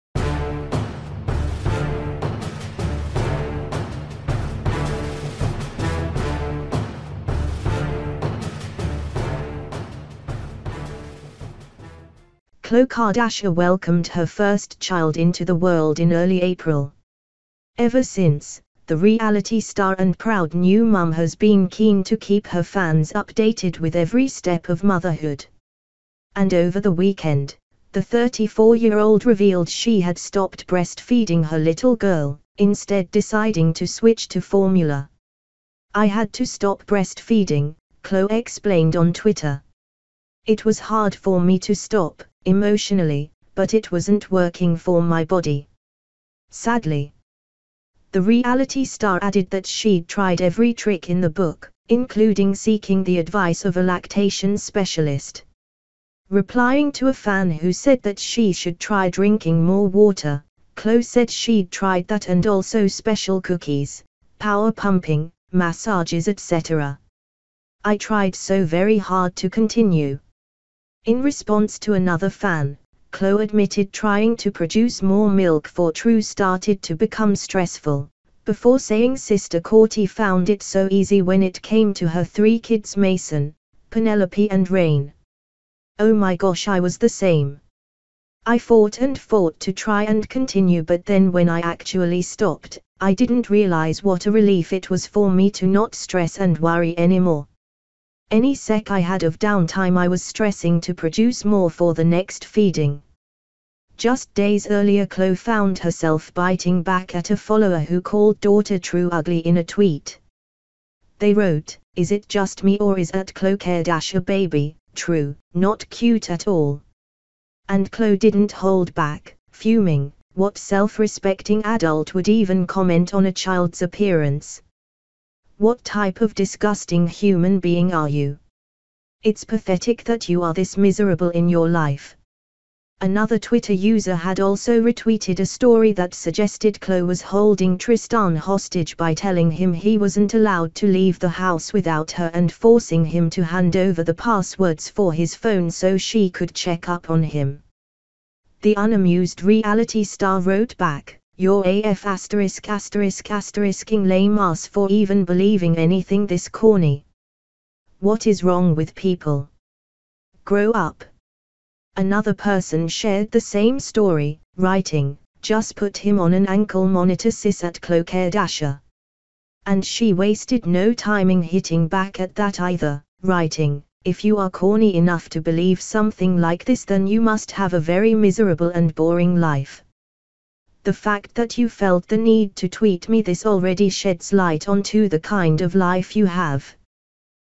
Khloe Kardashian reveals why she was forced to stop breastfeeding baby daughter True
watch the latest hollywood gossip and Sports news.